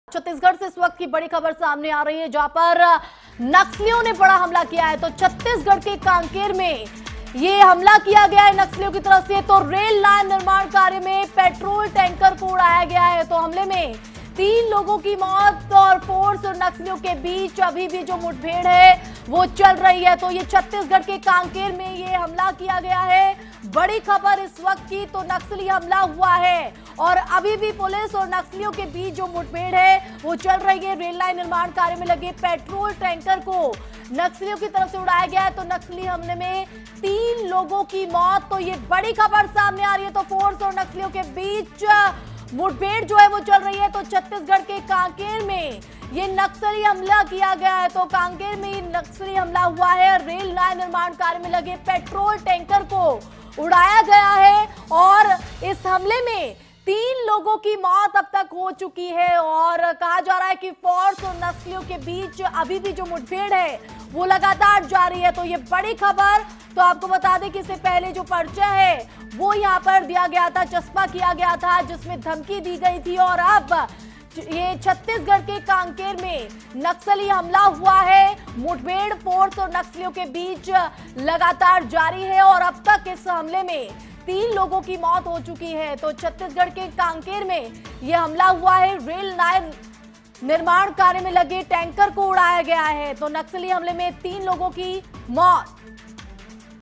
छत्तीसगढ़ में नक्सलियों का हमला
छत्तीसगढ़ के  कांकेर में नक्सलियों ने बड़ा हमला किया है...हमले में रेल लाइन निर्माण कार्य में लगे पेट्रोल टैंकर को उड़ा दिया है...हमले में 3 लोगों की मौत हो गई है...